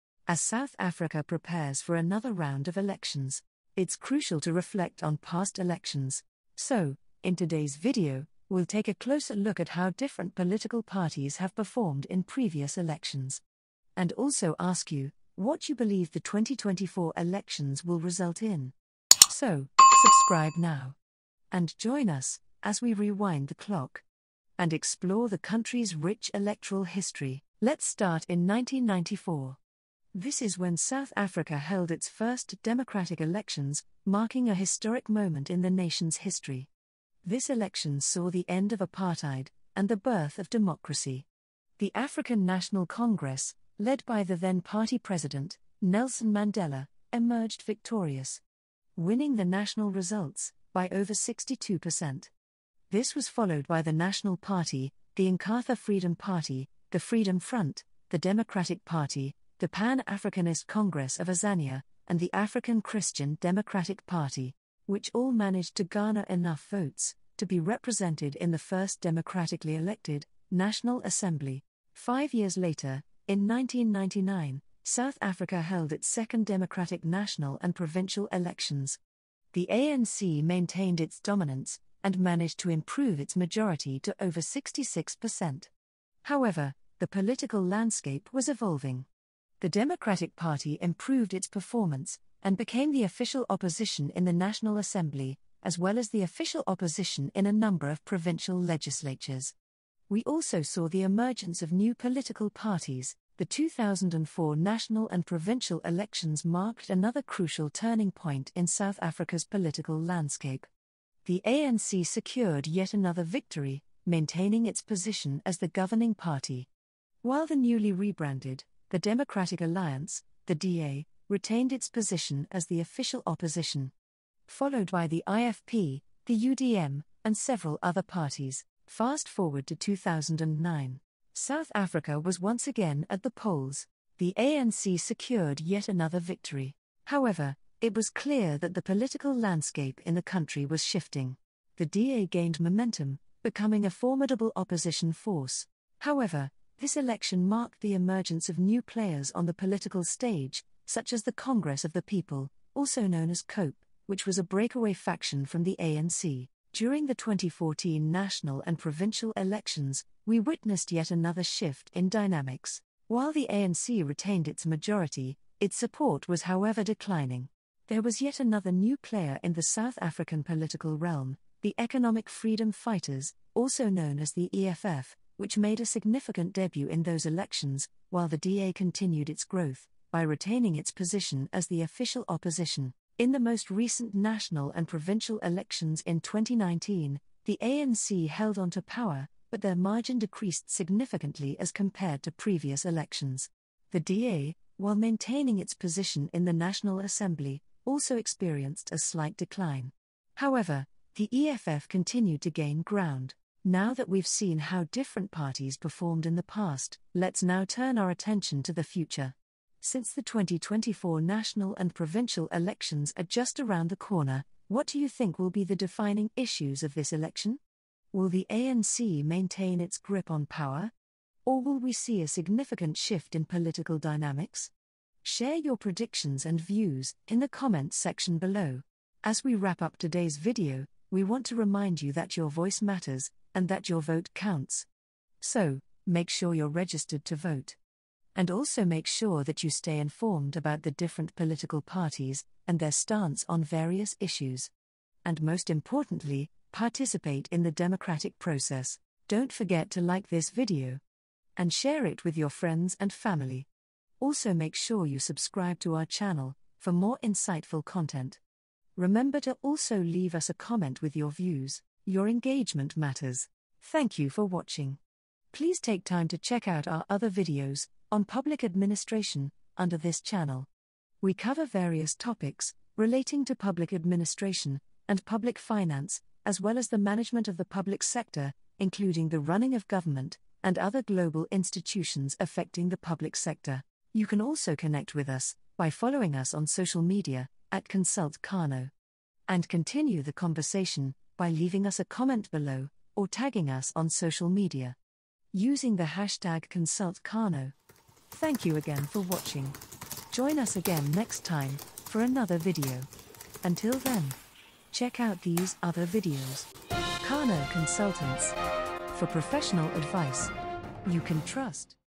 Election Results in South Africa From 1994 To 2019 | How Parties Performed
​In this video, we reflect on the share of votes received by certain South African political parties between 1994 and 2019.